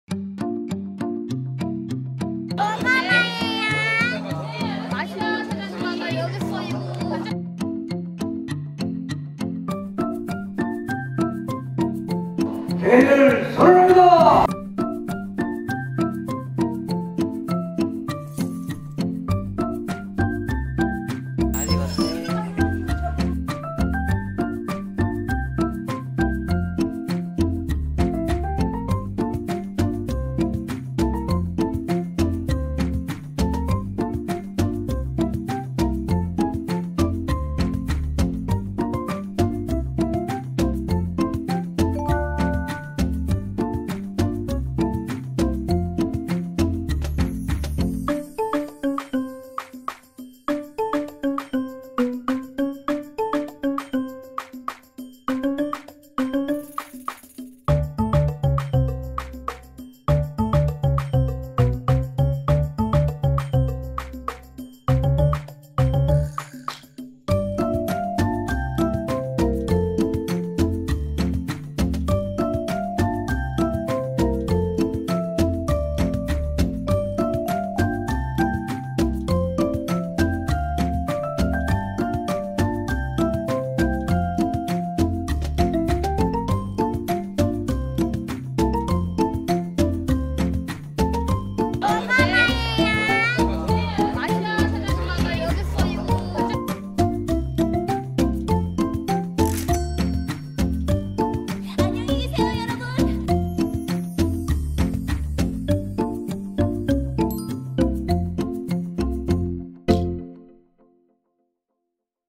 2024 해나루 황토 고구마 판촉행사 브이로그
지난 10월 5일 당진시 종합운동장에서 해나루 황토고구마 판촉행사가 있었습니다
끝없는 고구마들 중에 우리집에 갈 고구마는 어디있었을까요?
아직 집에 해나루 황토고구마가 없다구요?
당진팜 고구마 데려가세요!